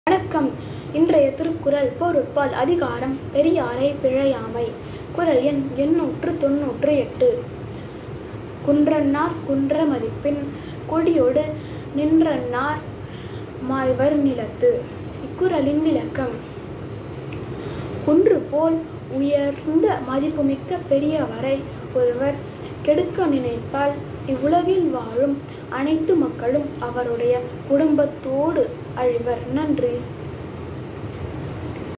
குறள் எண் - 898 திருக்குறள் - அதிகாரம் - பெரியாரைப் பிழையாமை - 90- பொருட்பால்
குன்றன்னார் குன்ற மதிப்பின் குடியொடு
நின்றன்னார் மாய்வர் நிலத்து.